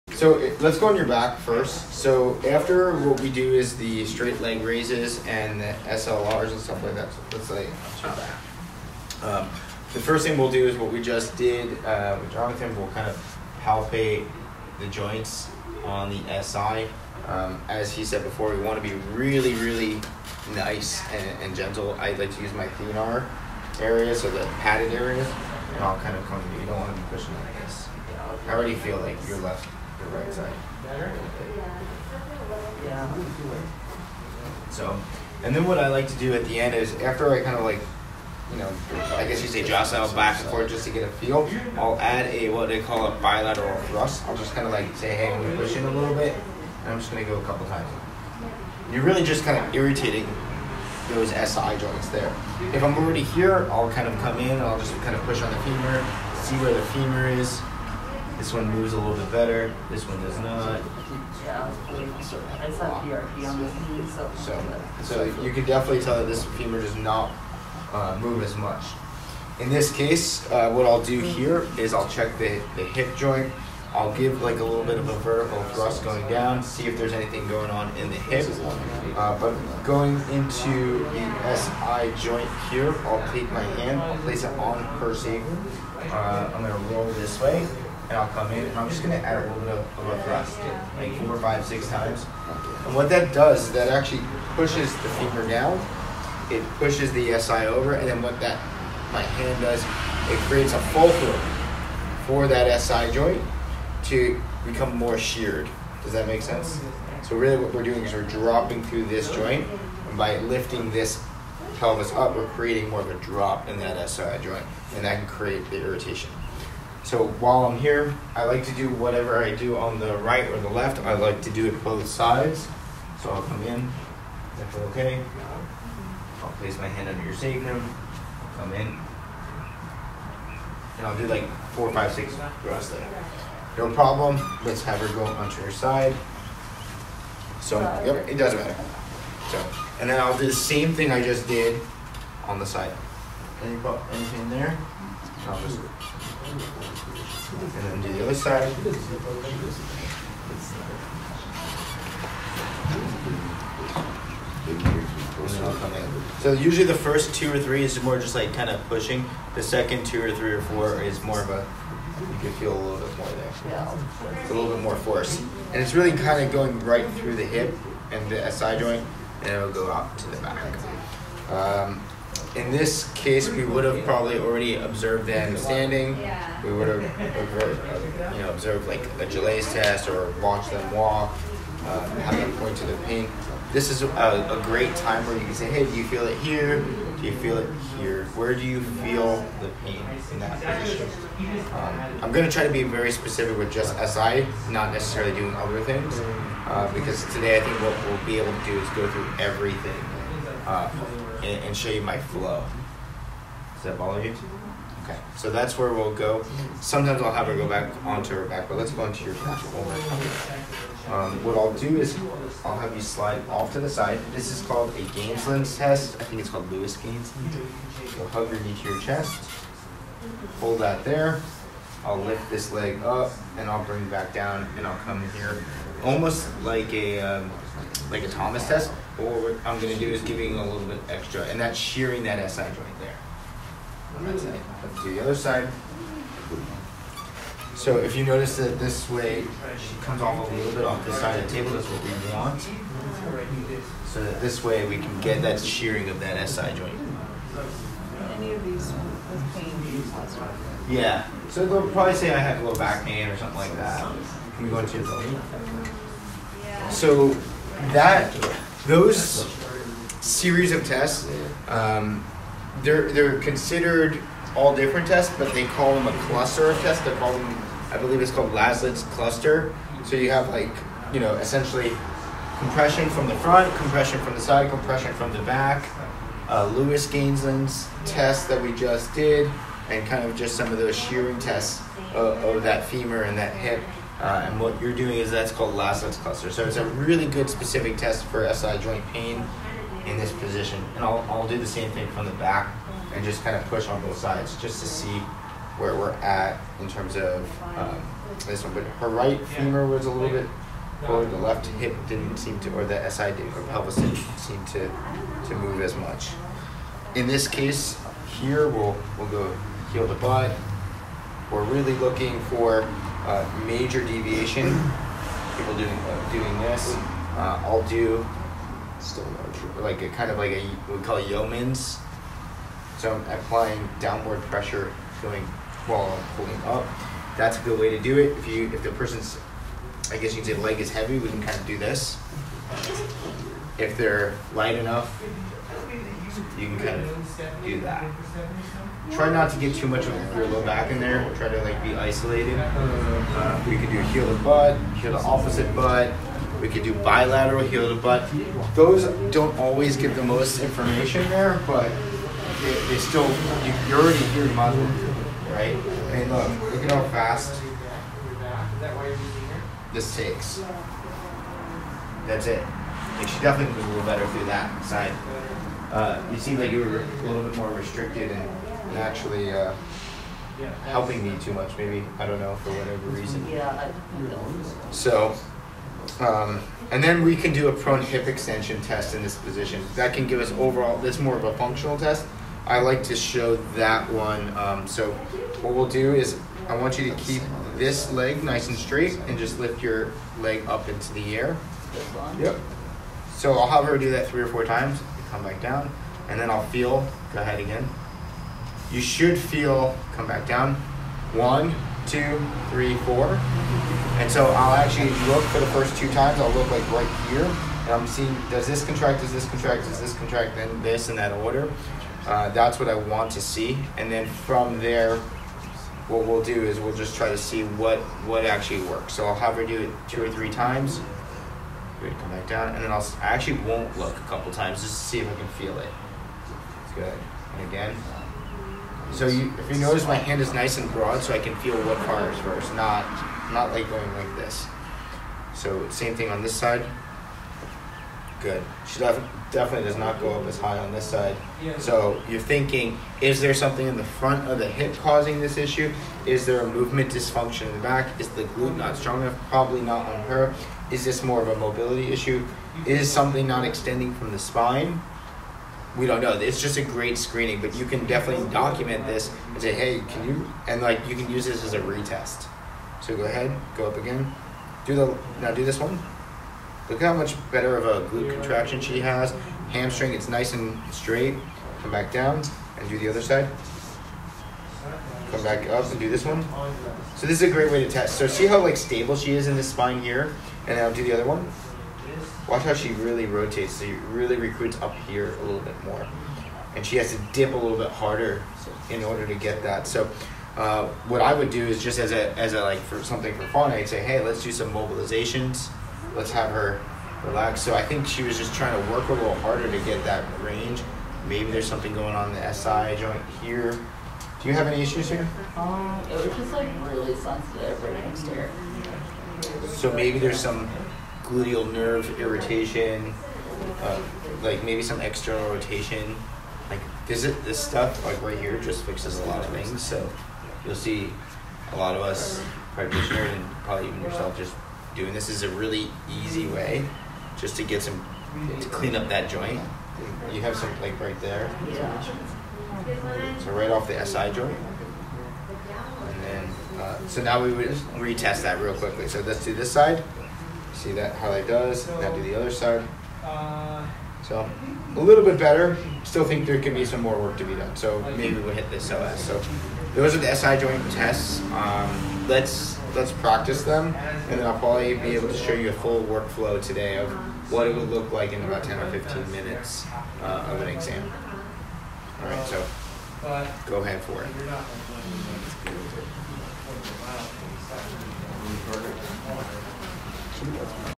Assessment of SI Joint Pain
Get the FREE Low Back Pain Relief Guide:
/low-back-pain-guideA lot of lower back pain can be caused by SI joint pain.  Here is an assessment I do for SI joint issues.